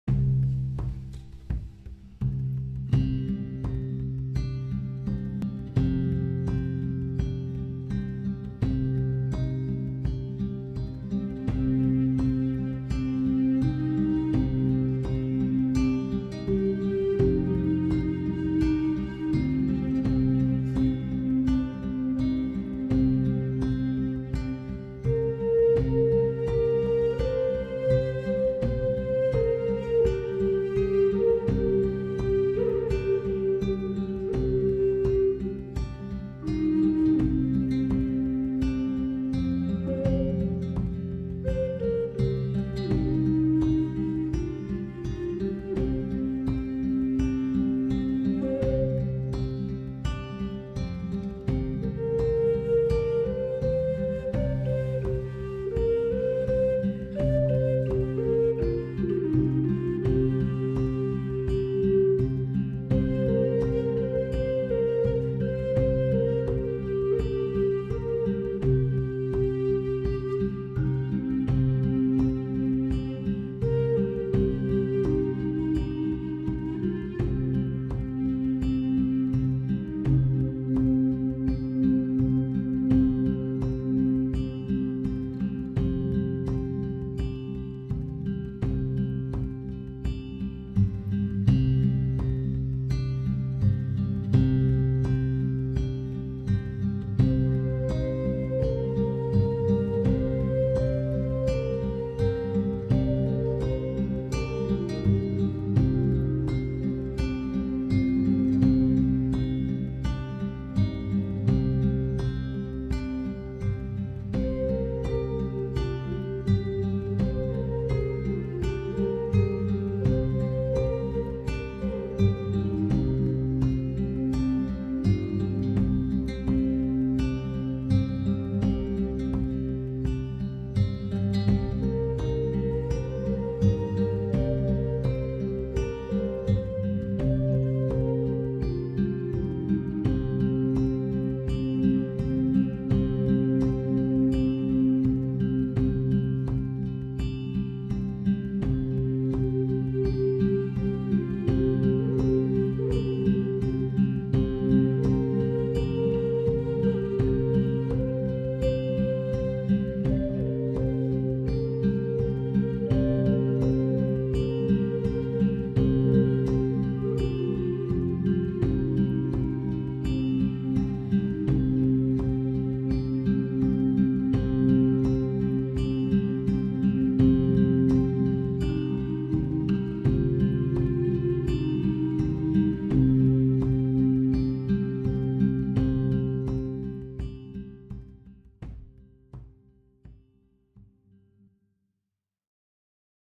Ditchbank Roads
Video of roads behind our summer home.   I am playing a C minor Jon Norris flute, and accompanied myself on guitar